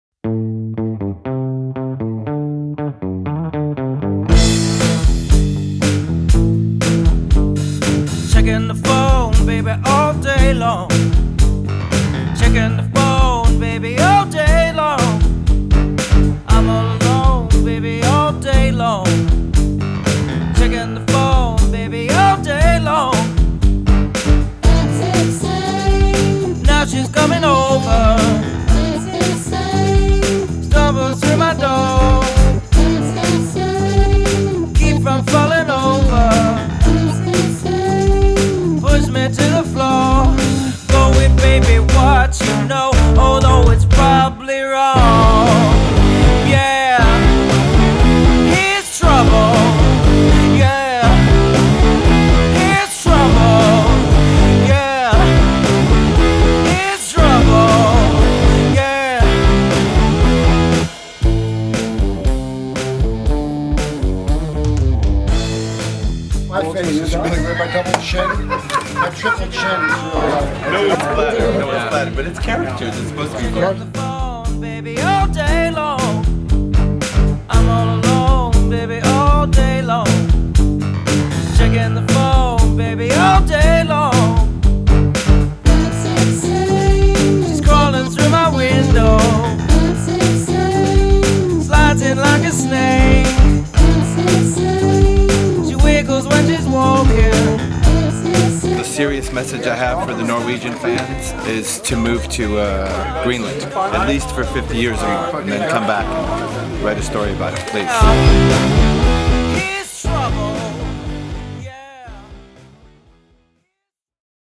Red Hot Chili Peppers & Burton snowboards meet & greet in Oslo!
"A serious message for the Norwegian fans" - Anthony Kiedis

Fast Forward management & event arranged a meet & greet session with Red Hot Chili Peppers and Burton snowboards in Oslo, Norway!

Check out exclusive Burton snowboards with RHCP graphics!